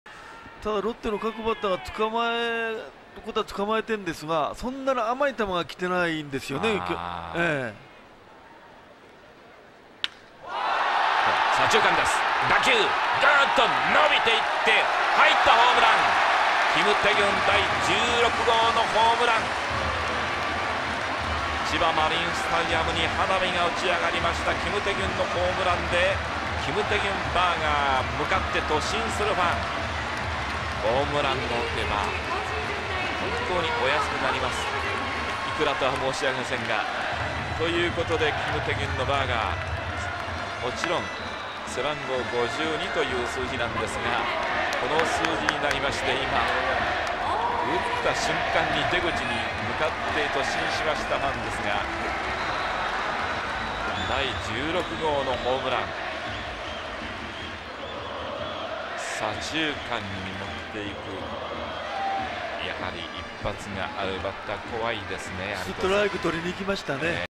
”軽く打ったように見えて…” 金泰均が豪快ホームラン
5回、千葉ロッテの金泰均が今季第16号ソロホームランを左中間へ放った。(2010年6月10日 千葉ロッテマリーンズ対広島カープ)